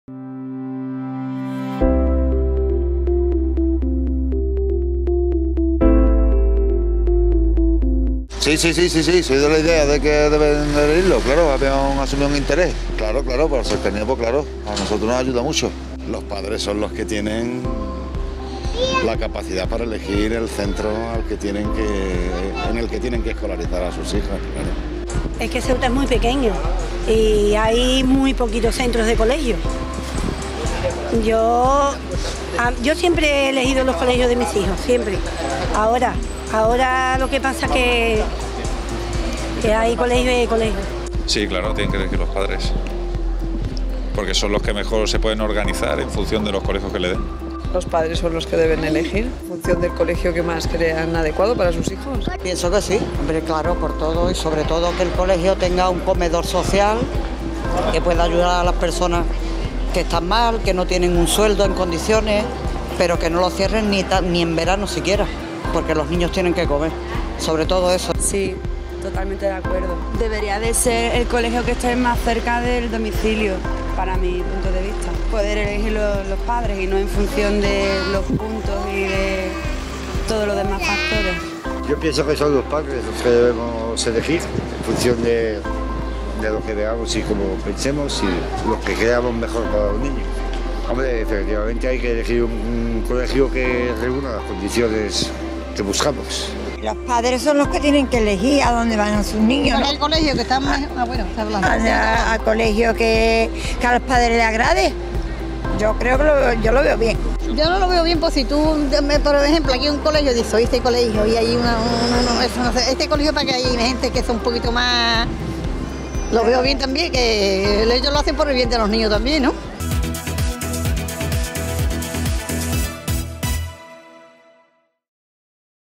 'La encuesta': ¿Deben los padres elegir el colegio para sus hijos?
En esta ocasión preguntamos a los ciudadanos a pie de calle su opinión respecto a la hora de matricular a sus hijos en un colegio público. Los ceutíes opinan para Faro TV, nuestra televisión